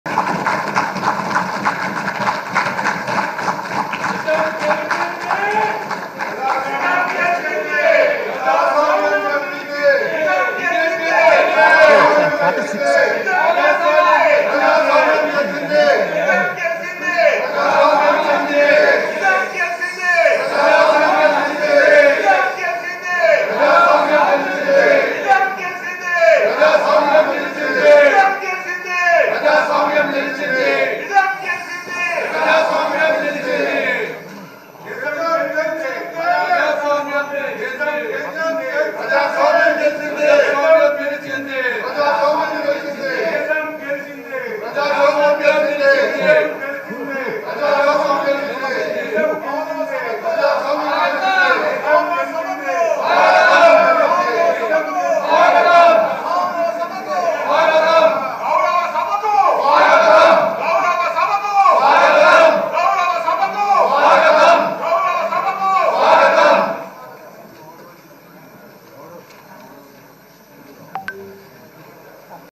అసెంబ్లీలో చంద్రబాబుకు గౌరవ స్వాగతం పలికిన సభ్యులు
రెండున్నరేళ్ల తర్వాత అసెంబ్లీలోకి అడుగుపెట్టిన ఏపీ సీఎం చంద్రబాబుకు (Chandrababu) శాసనసభ సభ్యులు గౌరవ స్వాగతం పలికారు. ‘నిజం గెలిచింది.. ప్రజాస్వామ్యం నిలిచింది’ అంటూ తెదేపా ఎమ్మెల్యేలు ప్లకార్డులు ప్రదర్శించారు. సభలో సీఎం చంద్రబాబు, డిప్యూటీ సీఎం పవన్‌ ఆలింగనం చేసుకున్నారు. గతంలో వైకాపా నేతలు తీవ్రంగా కించపరడంతో సీఎంగానే మళ్లీ అడుగుపెడతానని 2021లో చంద్రబాబు శపథం చేశారు. అది నేడు నెరవేరింది. దీంతో గౌరవసభకు స్వాగతం అంటూ తెదేపా ఎమ్మెల్యేలు నినాదాలు చేశారు.